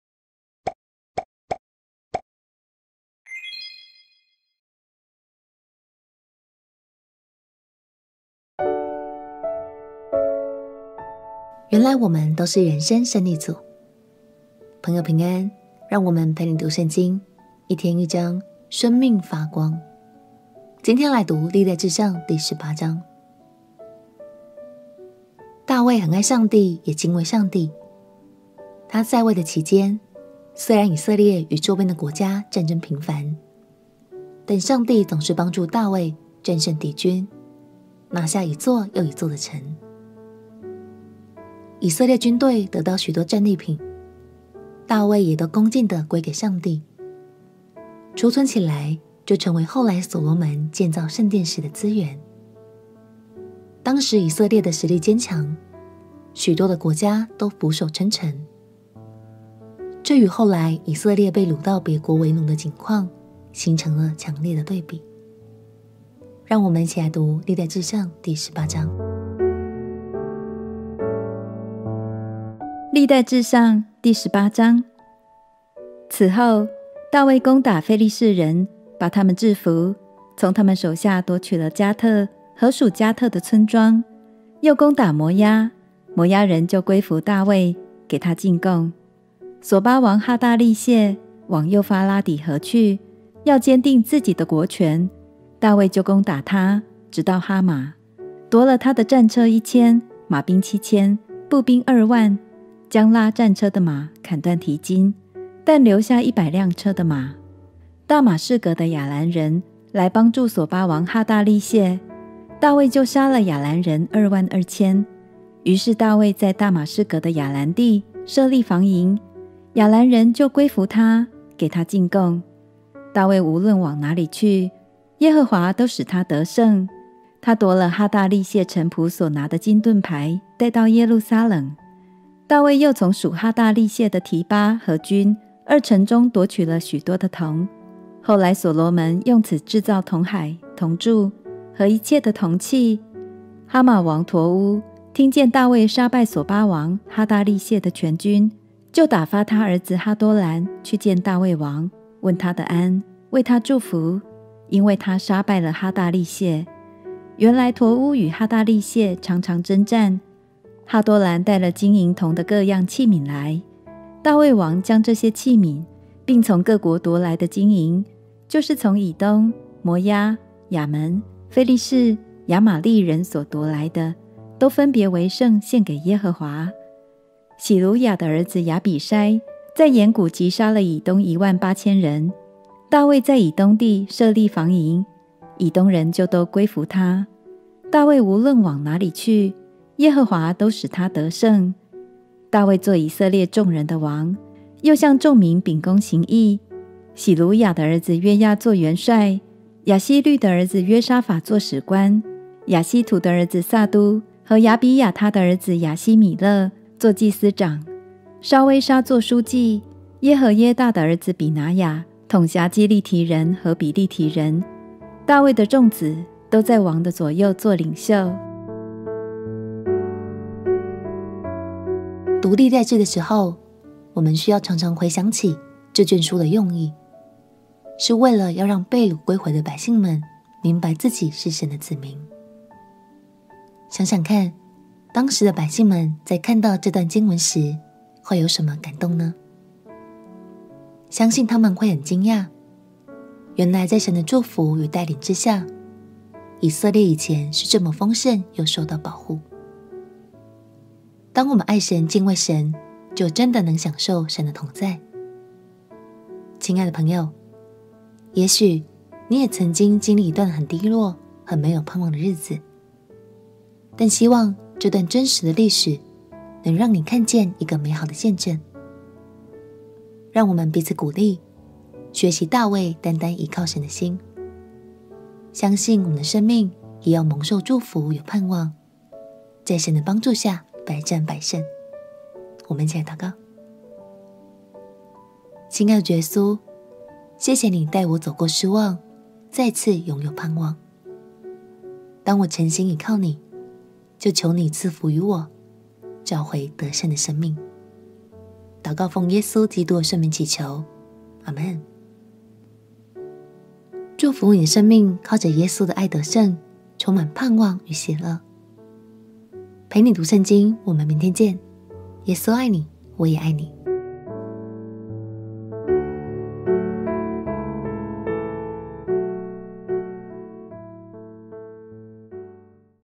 歷代志上18章/陪你讀聖經《原來我們都是人生勝利組！ 》
陪你讀聖經#356 歷代志上第十八章

原來我們都是人生勝利組！

朋友平安！讓我們《陪你讀聖經》一天一章 生命發光！今天來讀歷代志上第十八章

大衛很愛上帝，也敬畏上帝。他在位的期間，雖然以色列與週邊的國家戰爭頻繁，但上帝總是幫助大衛戰勝敵軍，拿下一座又一座的城。以色列軍隊得到許多戰利品，大衛也都恭敬地歸給上帝，儲存起來就成為後來所羅門建造聖殿時的資源。當時以色列的實力堅強，許多的國家都俯首稱臣，這與後來以色列被擄到別國為奴的景況，形成了強烈的對比...讓我們一起來讀歷代志上第十八章，請：

18:1此後，大衛攻打非利士人，把他們治服，從他們手下奪取了迦特和屬迦特的村莊；

18:2又攻打摩押，摩押人就歸服大衛，給他進貢。

18:3瑣巴王哈大利謝往幼發拉底河去，要堅定自己的國權，大衛就攻打他，直到哈馬，

18:4奪了他的戰車一千，馬兵七千，步兵二萬，將拉戰車的馬砍斷蹄筋，但留下一百輛車的馬。

18:5大馬士革的亞蘭人來幫助瑣巴王哈大利謝，大衛就殺了亞蘭人二萬二千。

18:6於是大衛在大馬士革的亞蘭地設立防營，亞蘭人就歸服他，給他進貢。大衛無論往哪裏去，耶和華都使他得勝。

18:7他奪了哈大利謝臣僕所拿的金盾牌帶到耶路撒冷。

18:8大衛又從屬哈大利謝的提巴和均二城中奪取了許多的銅。後來所羅門用此製造銅海、銅柱，和一切的銅器。

18:9哈馬王陀烏聽見大衛殺敗瑣巴王哈大利謝的全軍，

18:10就打發他兒子哈多蘭去見大衛王，問他的安，為他祝福，因為他殺敗了哈大利謝（原來陀烏與哈大利謝常常爭戰）。哈多蘭帶了金銀銅的各樣器皿來。

18:11大衛王將這些器皿，並從各國奪來的金銀，就是從以東、摩押、亞捫、非利士、亞瑪力人所奪來的，都分別為聖獻給耶和華。

18:12洗魯雅的兒子亞比篩在鹽谷擊殺了以東一萬八千人。

18:13大衛在以東地設立防營，以東人就都歸服他。大衛無論往哪裏去，耶和華都使他得勝。

18:14大衛作以色列眾人的王，又向眾民秉公行義。

18:15洗魯雅的兒子約押作元帥；亞希律的兒子約沙法作史官；

18:16亞希突的兒子撒督和亞比亞他的兒子亞希米勒作祭司長；沙威沙作書記；

18:17耶何耶大的兒子比拿雅統轄基利提人和比利提人。大衛的眾子都在王的左右作領袖。

讀《歷代志》的時候，我們需要常常回想起這卷書的用意，是為了要讓被擄歸回的百姓們明白自己是神的子民。想想看當時的百姓們在看到這段經文時，會有什麼感動呢？相信他們會很驚訝，原來在神的祝福與帶領之下，以色列以前是這麼豐盛又受到保護！當我們愛神、敬畏神，就真的能享受神的同在！親愛的朋友，也許你也曾經歷一段很低落、很沒有盼望的日子，但希望這段真實的歷史，能讓你看見一個美好的見證。讓我們彼此鼓勵，學習大衛單單倚靠神的心，相信我們的生命也要蒙受祝福有盼望，在神的幫助下百戰百勝！我們一起來禱告。

親愛的主耶穌，謝謝祢帶我走過失望，再次擁有盼望！當我誠心倚靠祢，就求祢賜福與我，找回得勝的生命！禱告奉耶穌基督的聖名祈求，阿們。